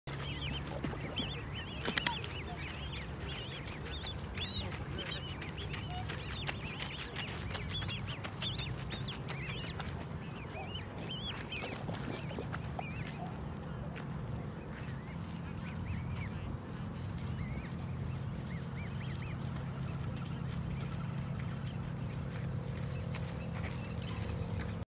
黒鳥親子♬ヒナの陸に上がる姿♬
黒鳥親子さん♪
陸に上がる姿が可愛いです😊

こちらもどうぞ😊↓
波が高い🌊上がれない💦頑張ってるヒナさん❣️黒鳥さん親子♡オーストラリア野生黒鳥。